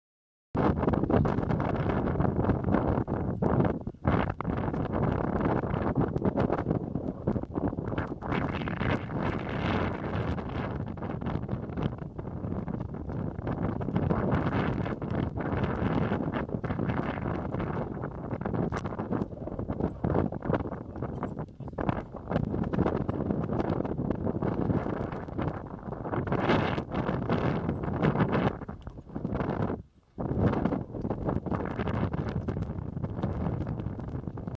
雪山の湯ノ丸山山頂から下山します
雪山の湯の丸山の山頂から早くも下山です。強烈な横風が吹いているので寒く退散です。
２０１３年２月９日撮影。

湯ノ丸山の雪山登山の詳細一覧はこちら。